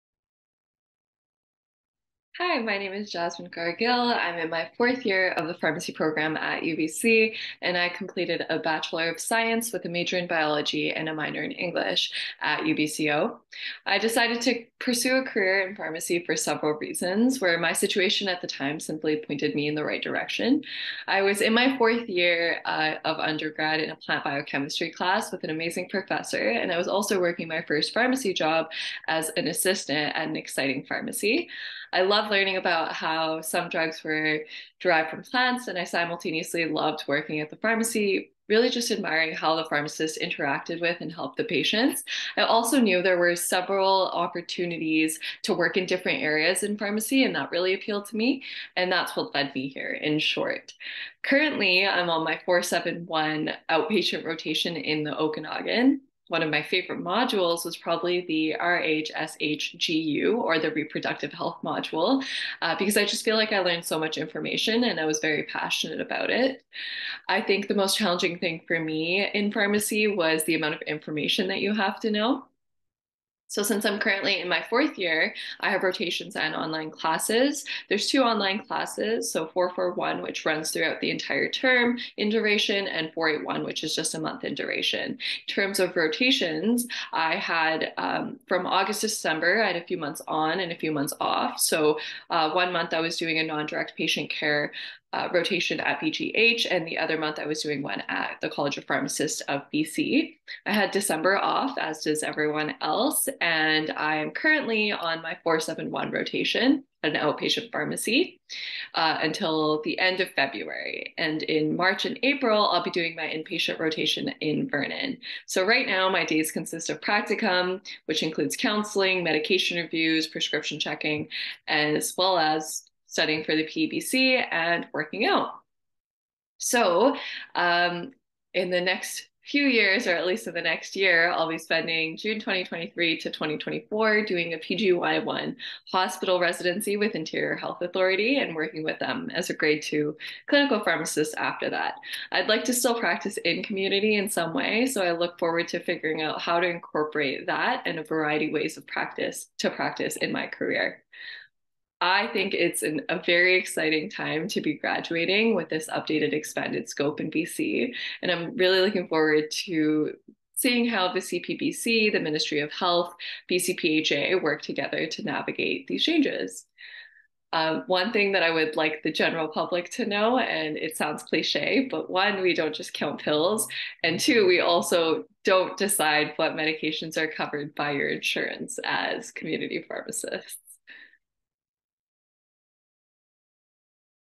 Jasmin Kaur Gill, fourth-year pharmacy student at UBC
Jasmin Kaur Gill, fourth-year PharmD student at UBC, talks about why she is pursuing a career in pharmacy. She answers some questions about what she's learning in pharmacy school.